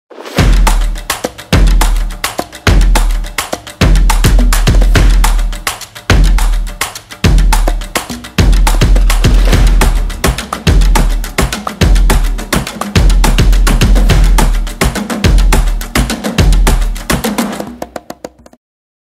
PhotoGrid Mixer & Pic Collage Maker - Photo Editor
PhotoGrid Mixer & Pic Collage Maker - Photo Editing! this is for photography and Instagram lover,
It's packed with features like scrapbook, face filter, meme, Instagram story, text, vintage, grunge, overlay and we have the most wonderful stickers, filters, patterns, frames, backgrounds a create beautiful photos.